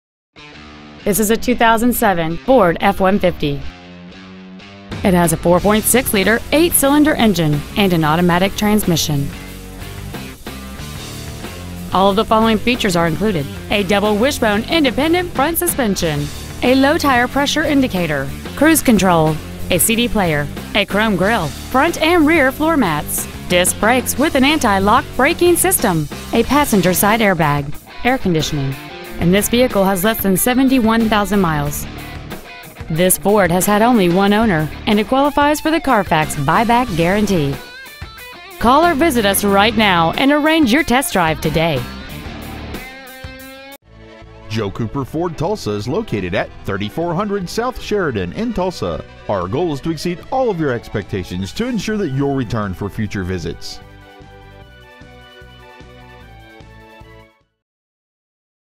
Used 2007 Ford F-150 Tulsa OK 74145
Year : 2007
 Make : Ford
 Model : F-150
 Engine : 4.6L EFI V8 engine
 Trans . : Automatic
 Exterior : Blue
 Miles : 69,664

 Stock : CC76944